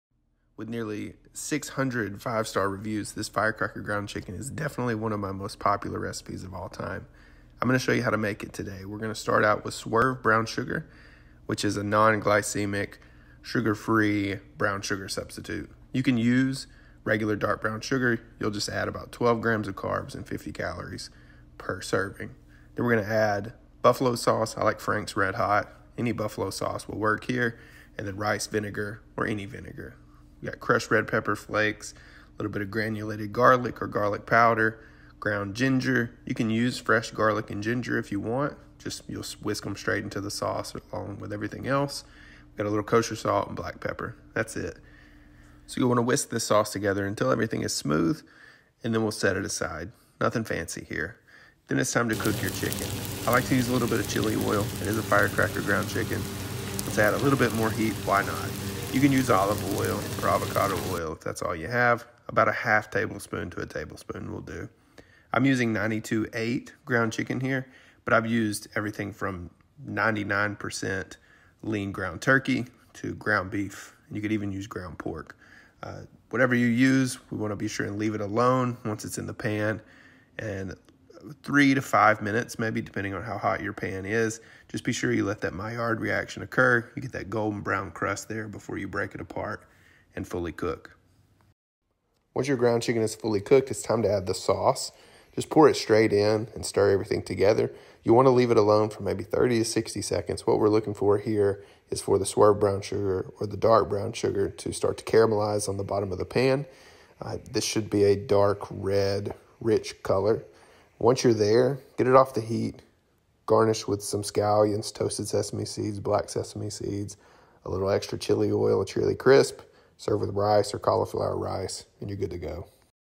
Quick And Easy Firecracker Groung Chicken Recipe Ever
Ingredients
 Everything you’ll need for the firecracker sauce:
* buffalo sauce
* cider or rice vinegar
* zero-calorie Swerve brown sugar (or dark brown sugar)
* garlic powder
* ground ginger
Simply mix everything together and you’ll be ready to crack on.

Everything you’l need for the Chicken:
* 1 lb Ground Chicken (92/8)
* 1/2 Tbsp (8g) Chili Oil
* 1/2 C (96g) Swerve Brown Sugar
* 1/4 C (60g) Buffalo Sauce
* 2 Tbsp (30g) Cider or Rice Vinegar
* 1 tsp Ground Ginger
* 1 tsp Garlic Powder
* 1/2 tsp Kosher Salt
* 1/2 tsp Black Pepper
* 1/2 tsp Red Pepper Flake

Instructions
1. Heat a skillet over medium-high heat with the chili oil. Brown both sides of the chicken, about 3-4 minutes per side, before mincing and fully cooking.
2. While the chicken cooks, whisk the remaining ingredients together in a mixing bowl.
3. Once the chicken is fully cooked, add the sauce to the pan and cook until thick and syrupy.
4. Serve with scallions and toasted sesame seeds over rice or cauliflower rice.